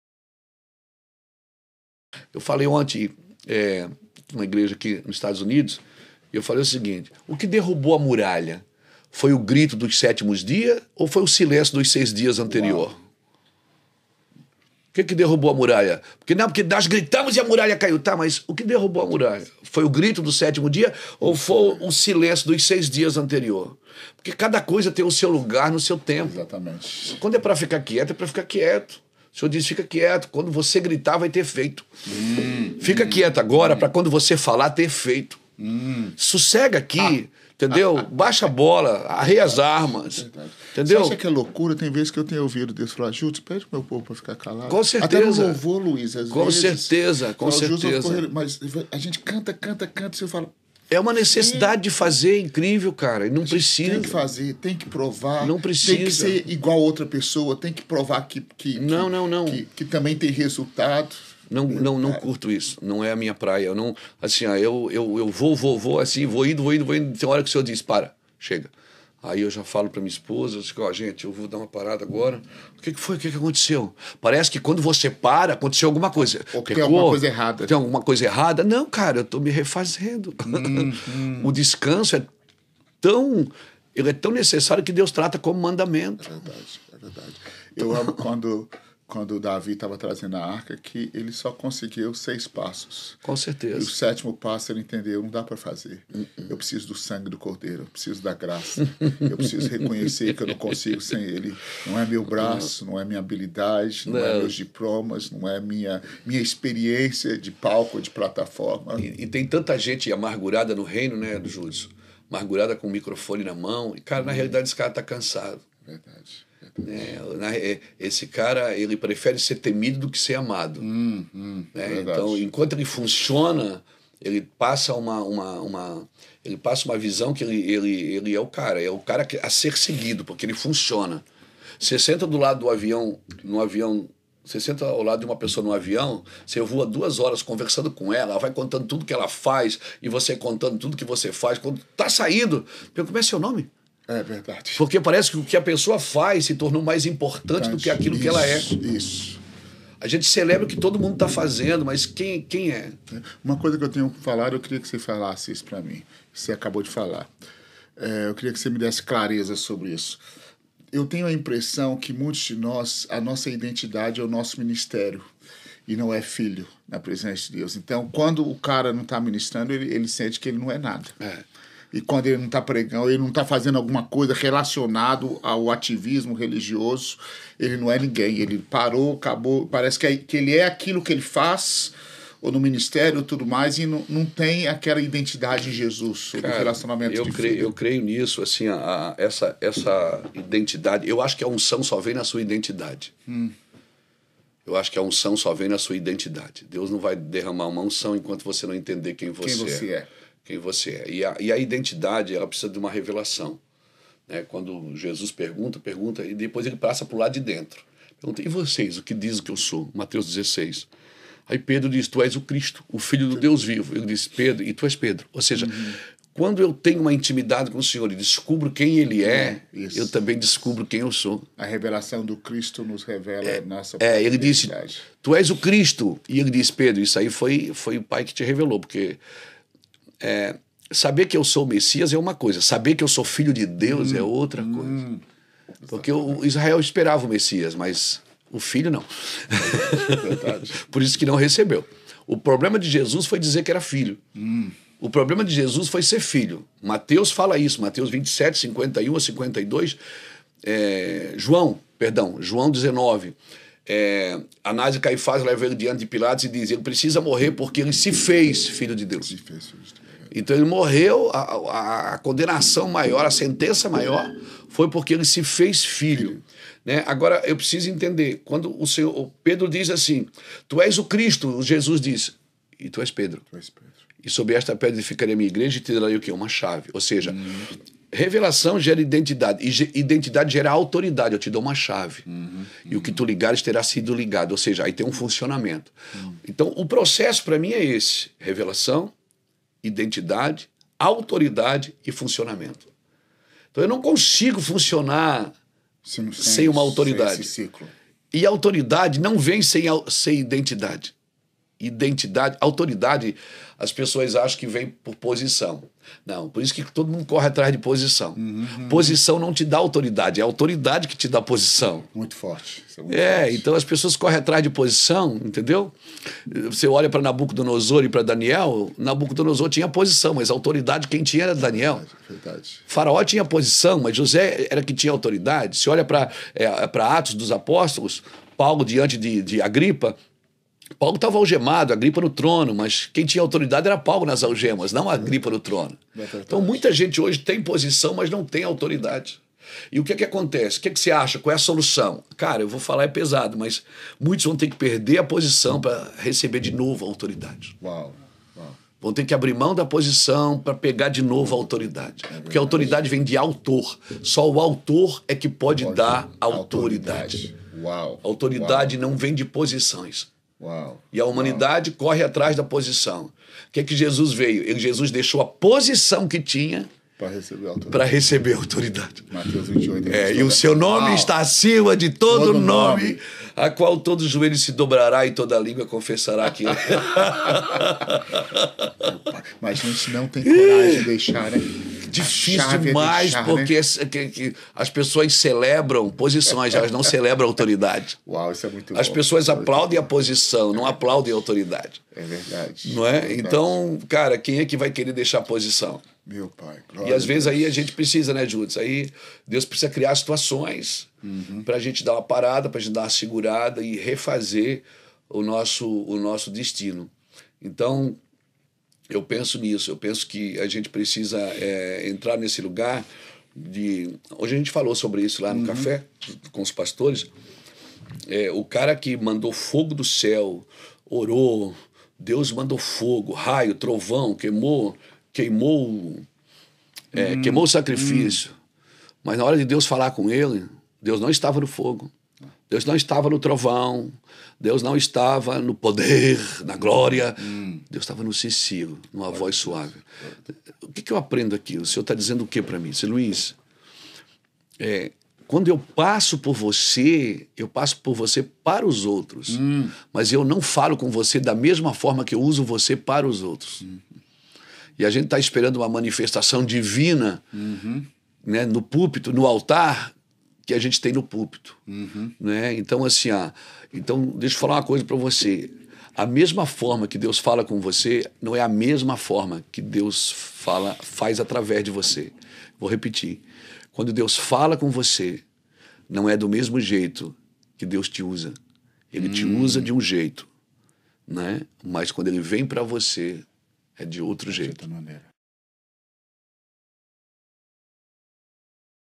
Cortes do Judson de Oliveira e Luiz Herminio
Uma pequena porção da palavra de forma reveladora.
Isso acontece quando dois homens de Deus se encontram